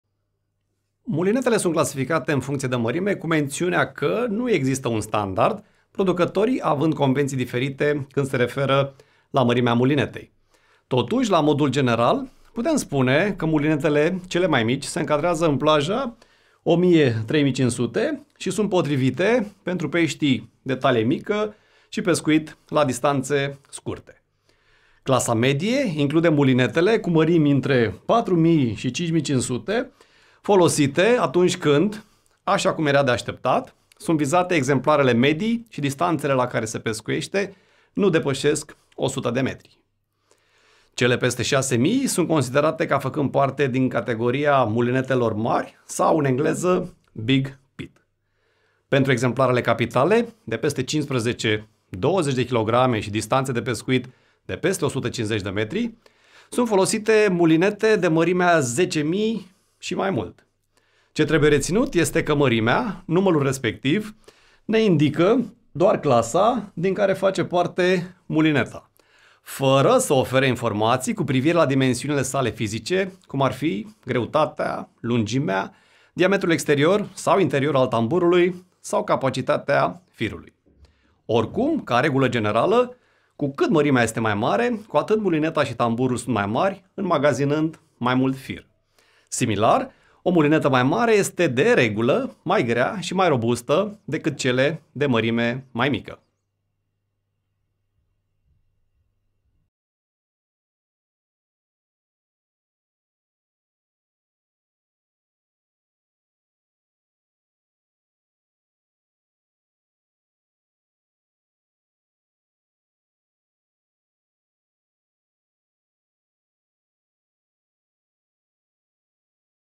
Pescuitul crapului | Mărimea mulinetei
Mărimea mulinetei nu este un parametru standard, notația - cifra care indică mărimea - putând să difere de la un producător la altul. Deși există câteva convenții general acceptate prezentate în materialul de față, pescarul trebuie să se asigure că mulineta pe care o vizează este de  mărimea pe care o dorește.

În acest capitol:
- clasificarea mulinetelor în funcție de mărimi
- alegerea mărimii potrivite
- relația dintre mărimea mulinetei și dimensiunile fizice.